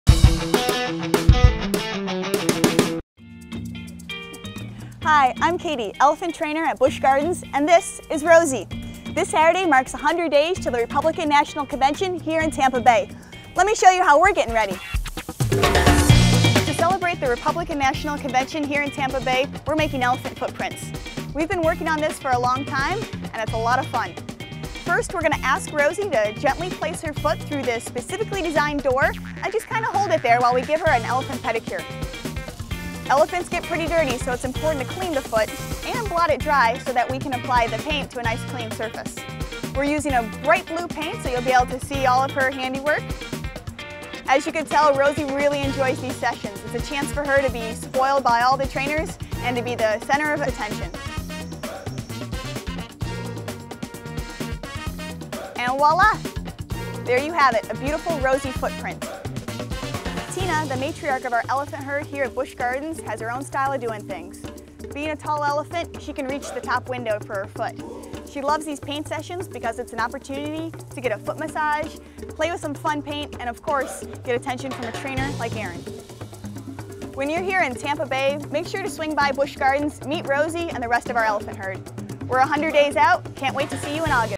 Creating elephant footprints at Busch Gardens Tampa for the Republican National Convention
Busch Gardens' elephant team added some "colorful" enrichment to the elephants' day by making "elephant footprints" with non-toxic children's poster paint at the park's new Elephant Interaction Wall. The unique prints will be framed and shared with special guests and members of the media who visit Busch Gardens during the Republican National Convention.